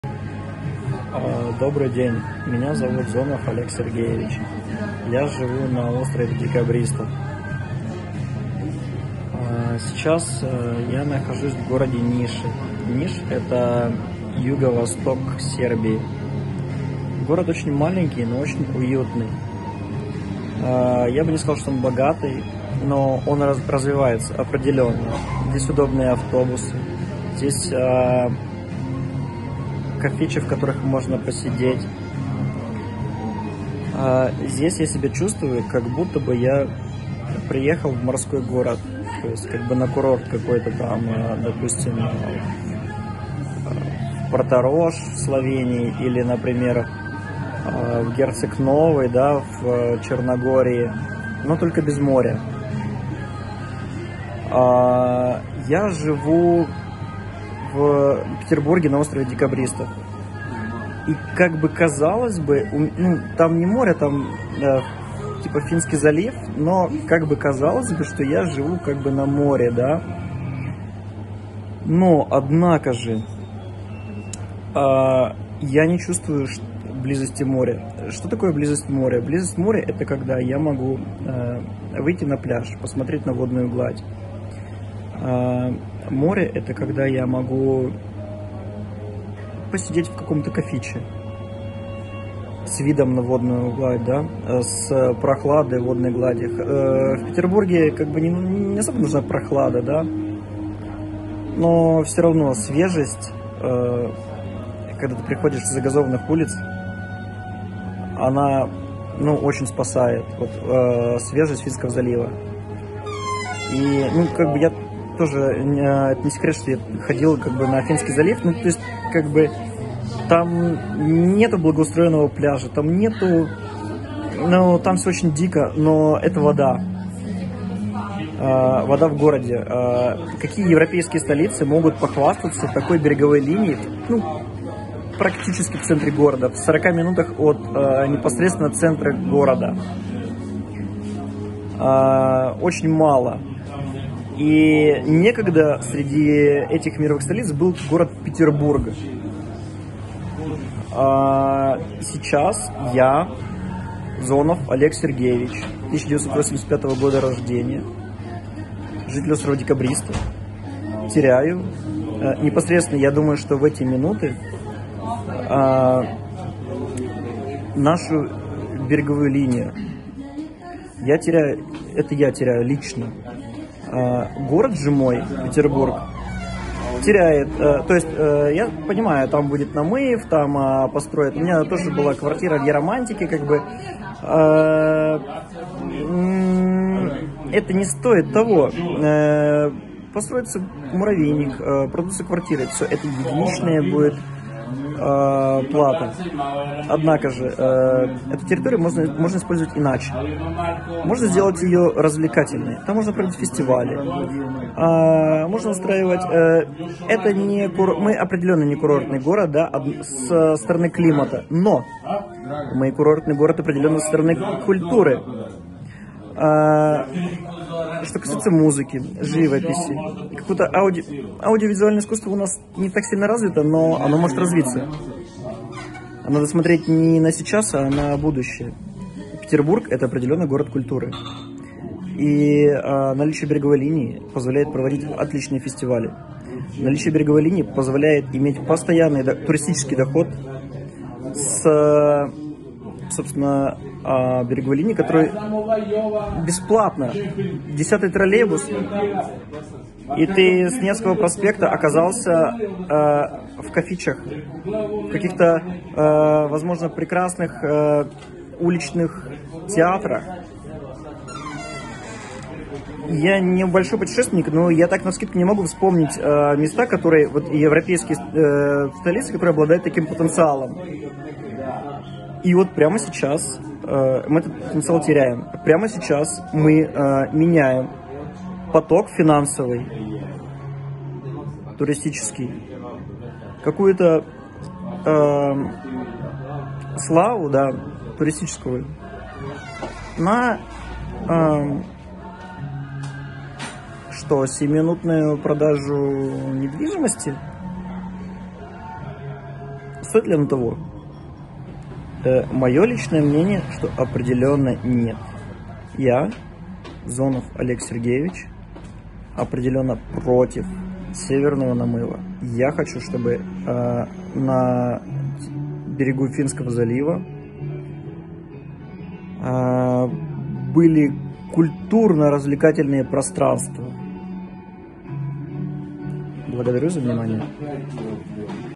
Санкт-Петербург Потенциал города За пляж Против серверного намыва Остров Декабристов
Альтернативное беспорядочной застройке мнение о правильном развитии комфортной городской среды Видео обращение Гражданская позиция